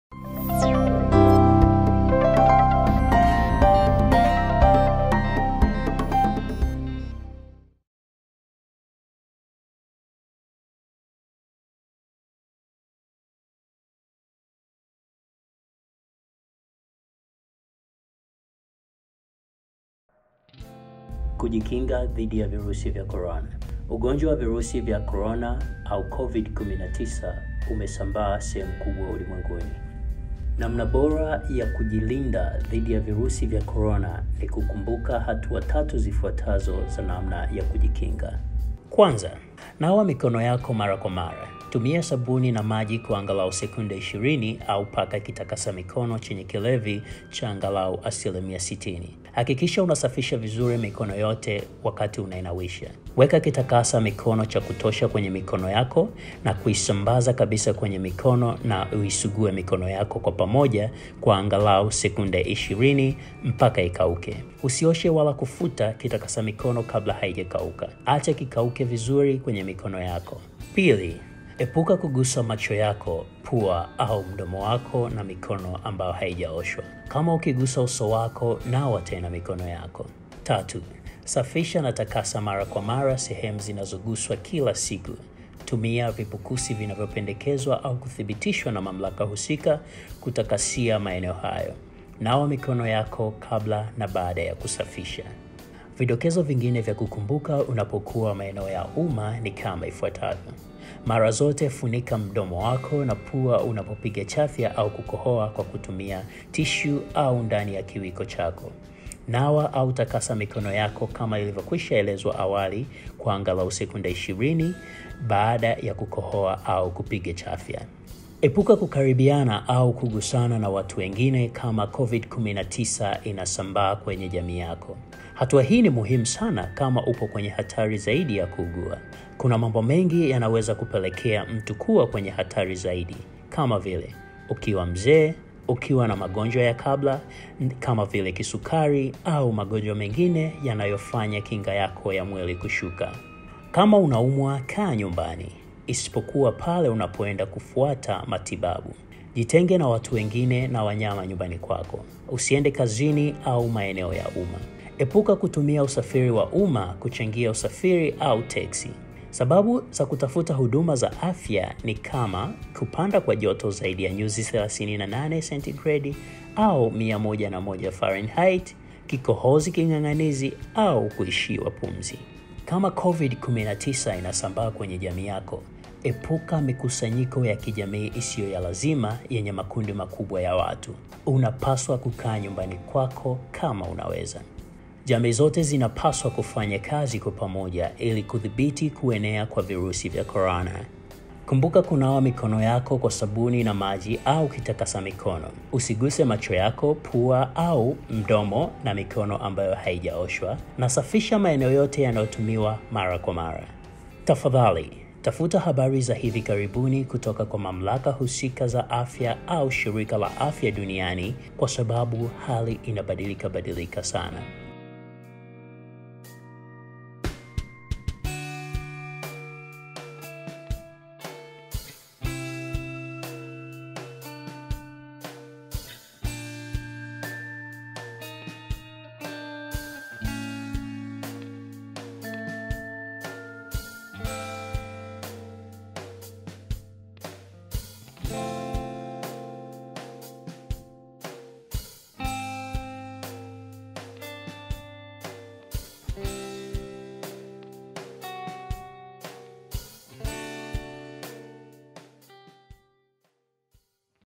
Kujikinga Dhidi ya Virusi vya Korona Kwa Kiswahili (Lafudhi ya Tanzania)
Ugonjwa wa virusi vyajo korona au COVID-19 (Kumi na tisa ), Umeshasambaa shemu kubwa ulimwenguni.  Kumbuka kunawa mikono yako kwa sabuni na maji au kitakasa mikono, Usiguse macho yako pua au mdomo na mikono ambayo haijaoshwa, na safisha maeneo yanayotumiwa mara kwa mara.

Protecting Yourself from Coronavirus in Swahili (accent from Tanzania)

COVID-19 has now spread around most parts of the world. This animation addresses best practices to protect against Coronavirus through recommended preventative measures.